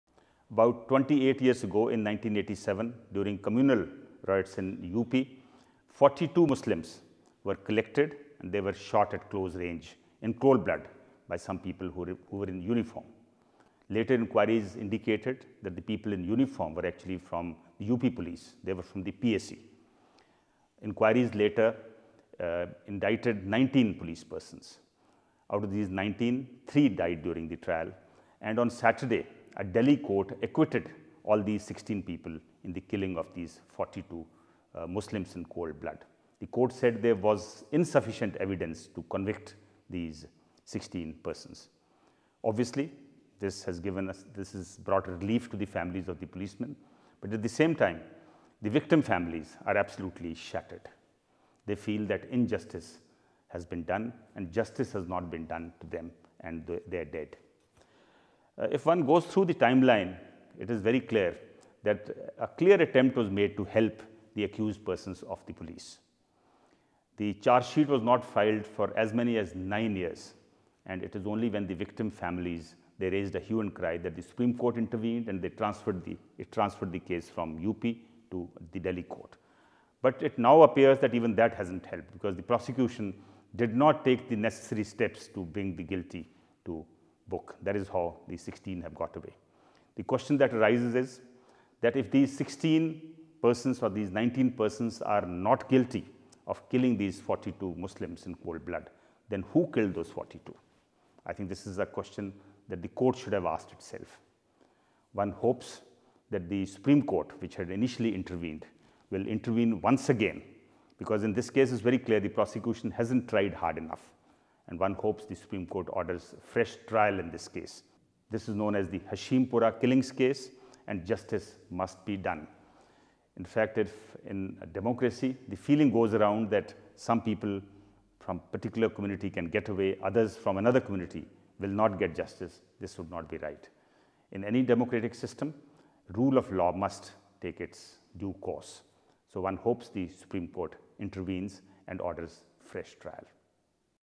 Need for fresh trial of Hashimpura killings (English)
The Court decision yesterday on the Hashimpura killings in UP in 1987 exposes weaknesses of the Indian justice system. During the communal riots 28 years ago, 42 Muslims were rounded up and shot in cold blood and their bodies thrown in canals by certain people in uniform. Subsequent inquiries indicated that the uniformed persons were personnel of the PAC, a police force of UP. On Saturday, a Delhi Court acquitted all the 16 accused (3 of the 19 accused had died during trial), giving them the benefit of doubt. While this has given the accused a sense of relief, it has angered the victims' families no end. The Court also referred the case to a legal authority for rehabilitation of the victims.

There were efforts to save the accused from the very beginning. Imagine the charge-sheet in the case was filed after nine years. In fact, the case progressed only after the Supreme Court intervened on plea of the victim families and transferred the case from UP to Delhi. Clearly this has also not helped as it appears the prosecution did not try hard enough to pin down the accused. This case should in fact have been a test case and not treated lightly. The very fact that the Court has referred the case for rehabilitation of the victim families shows that the court does accept this to be a case of custodial killing. The question that arises is: Who killed the 42 persons if not these accused persons of the PAC? One hopes the Supreme Court takes stock, raises vital questions and refers the case back for retrial. Purported injustice is such cases often gives the victims reasons to take law in their own hands, which, when it happens, is a blot on democratic functioning.